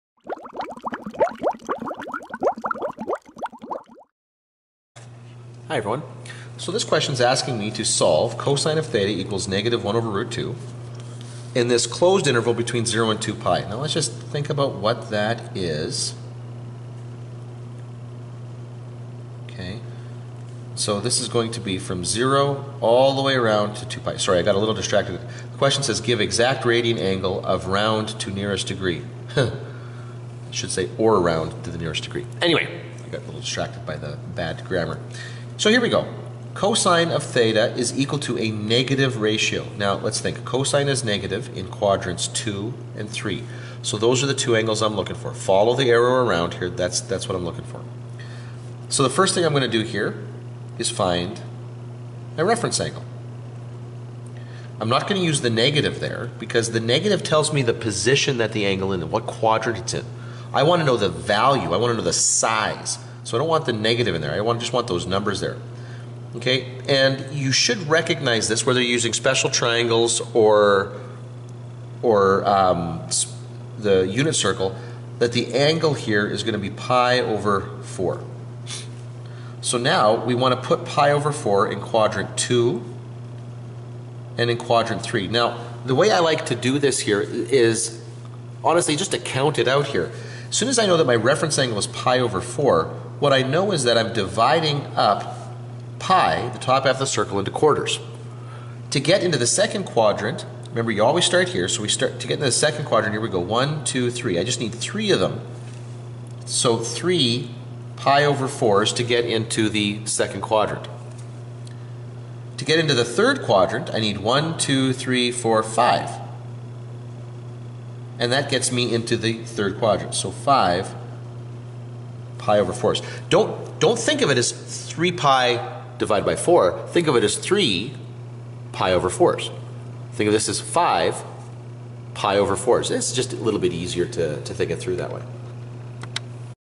Math 30-1 Trig I Review
This video is about solving a trig equation.